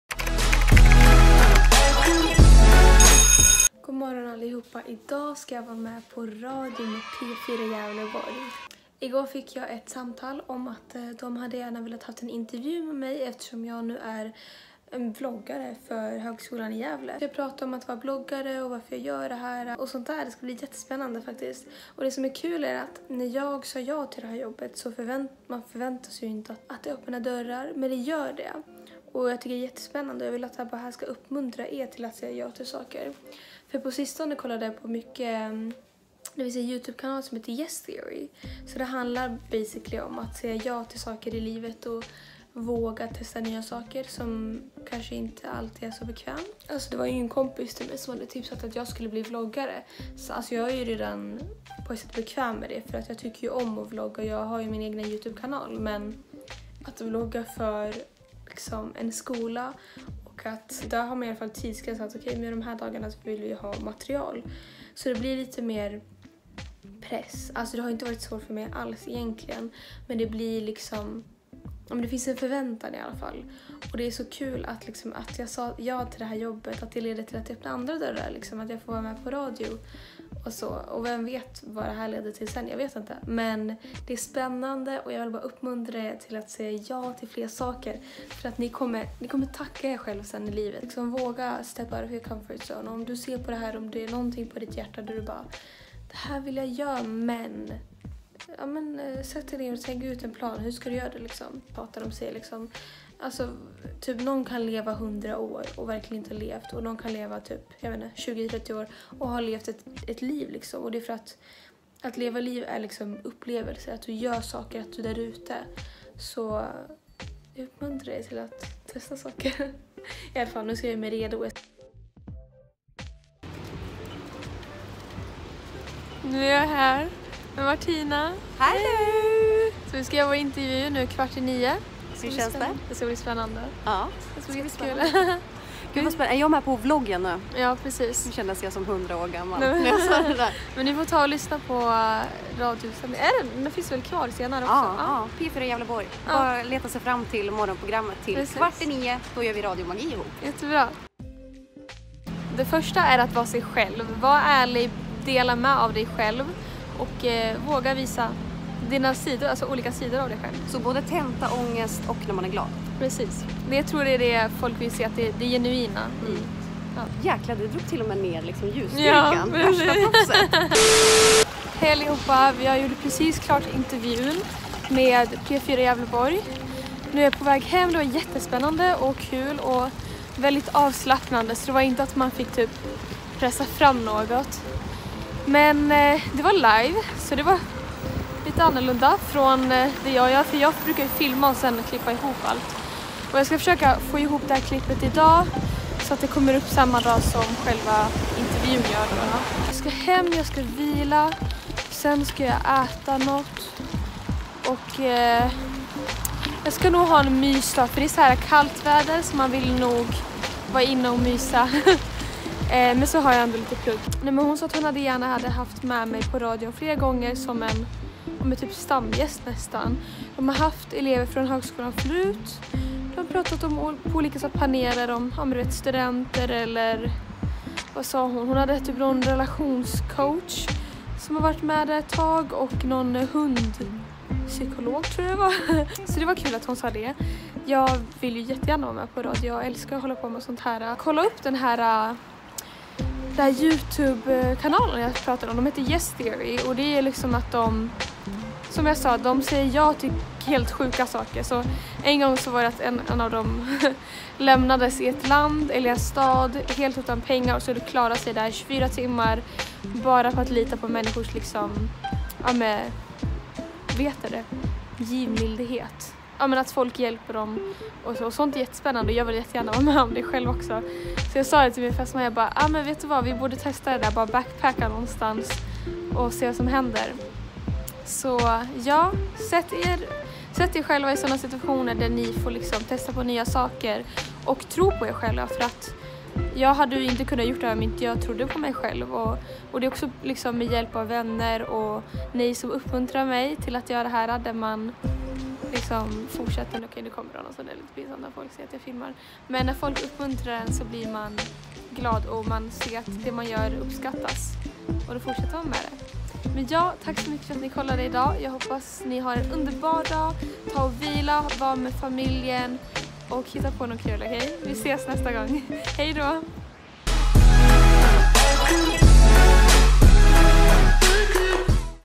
BONUSVLOGG: Sando gästar P4 Gävleborg
Sando gästar P4 Gävleborg och pratar om sitt vloggande för Högskolan.

Sando Raouf, student vid Högskolan i Gävle och Högskolans officiella vloggare sen hösten 2019.